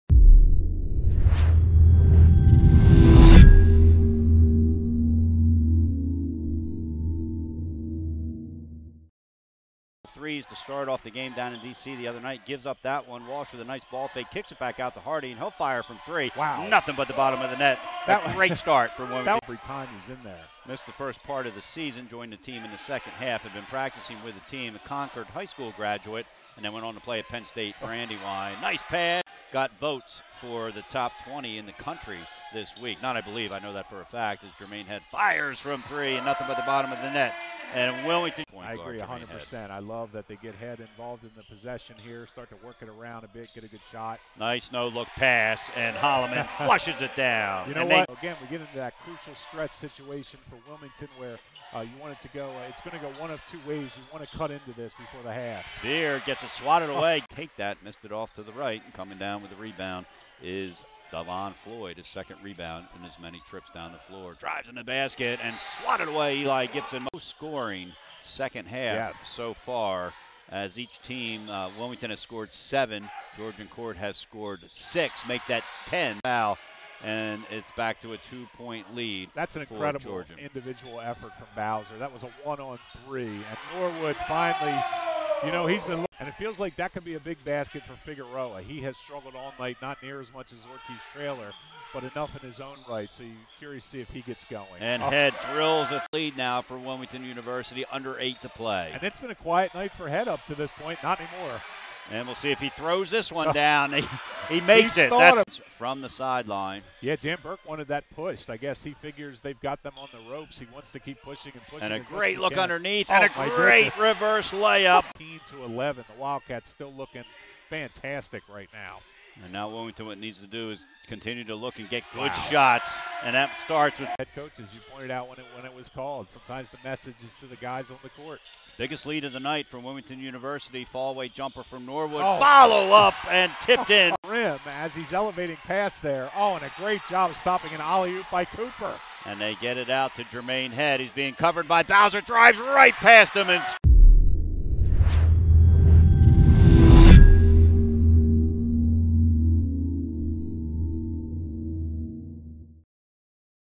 Men's Basketball vs. Georgian Court
The Wilmington University men's basketball team defeated CACC foe Georgian Court, 76-66 Wednesday night at the WU Athletics Complex. The men would go on a 30-9 run against the Lions in a span of eight minutes to control the lead. Jermaine Head had a game-high 32 points, with 6 rebounds, and 6 assists, while going 8-8 from the free throw line. Jordan Harding added 11 points and 6 rebounds, as Kameron Cooper chipped in with 9 points. Freshman Eli Gibson grabbed 9 rebounds. WU is now 4-10, 2-3 CACC.